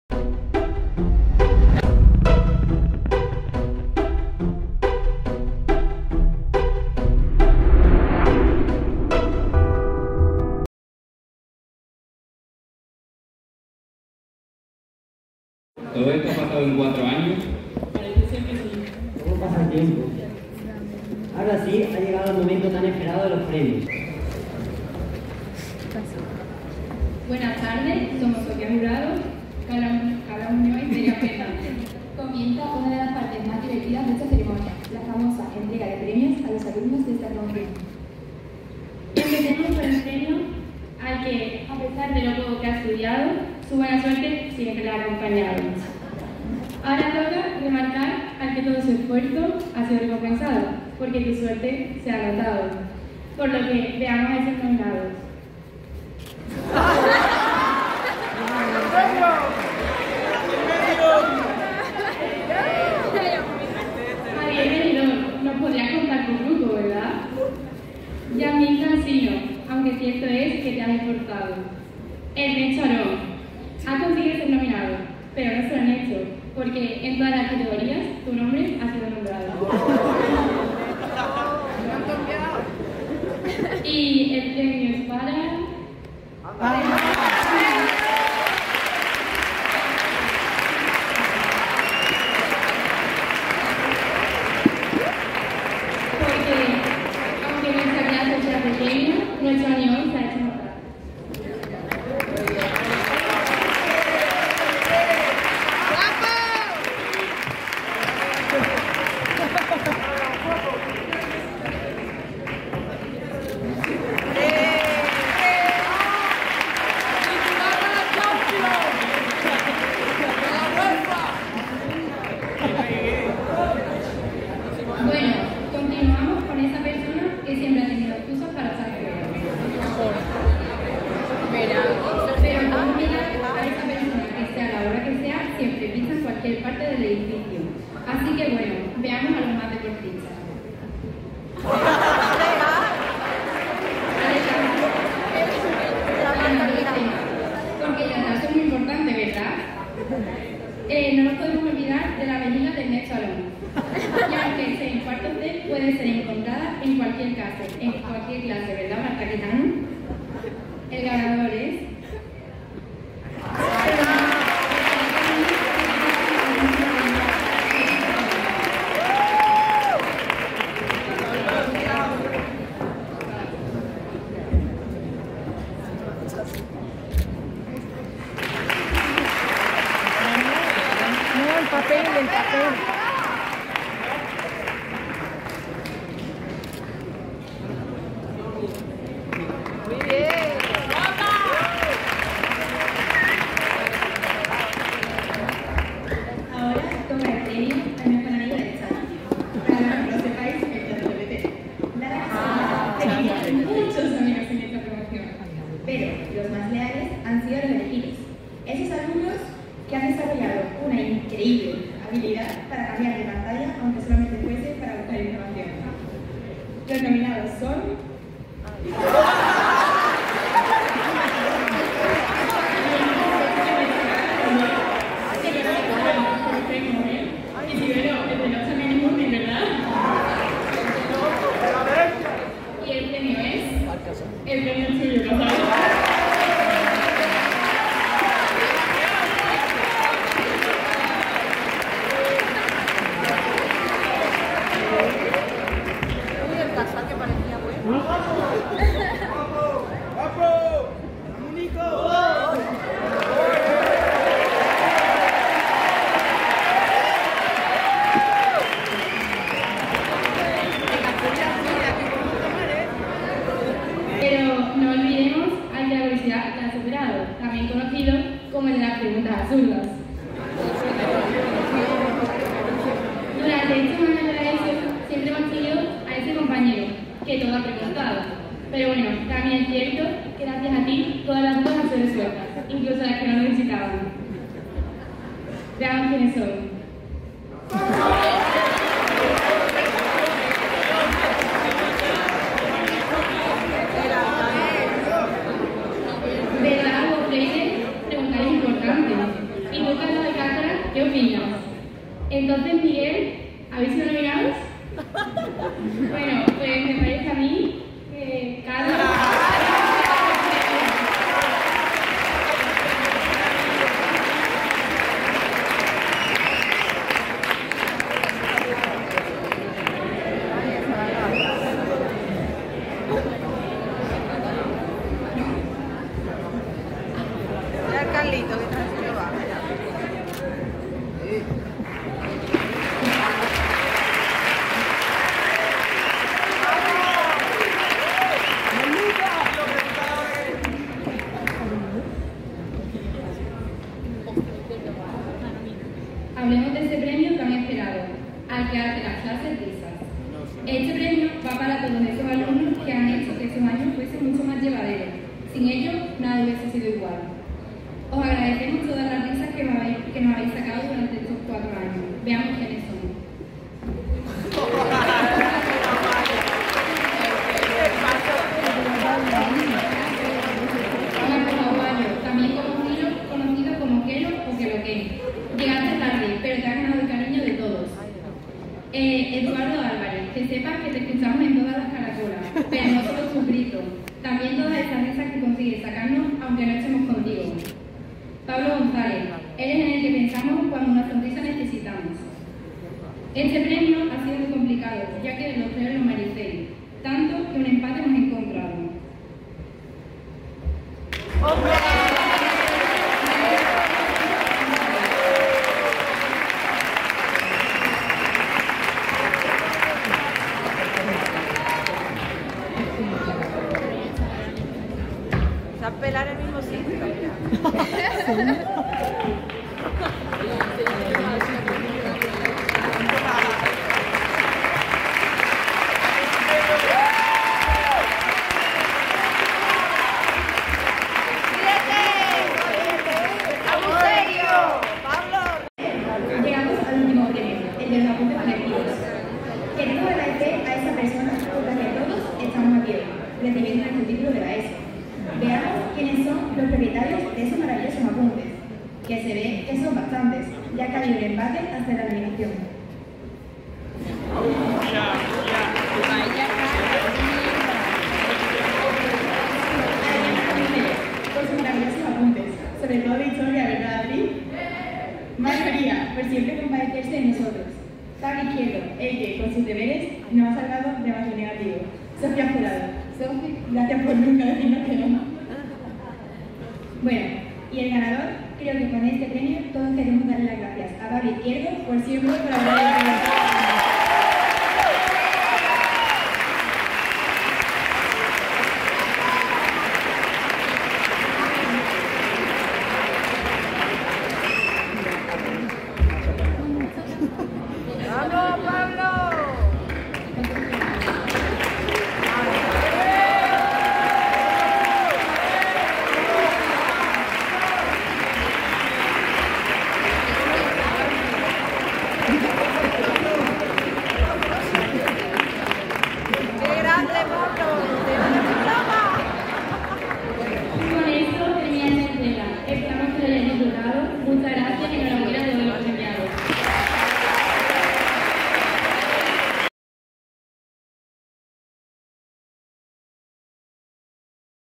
GRADUACIÓN 4º DE ESO. LA ENTREGA DE LOS DIVERTIDOS PREMIOS AL ALUMNADO.
La entrega de los divertidos premios al Alumnado, en el acto de GRADUACIÓN DE LA 10º PROMOCIÓN DE 4º DE LA ESO.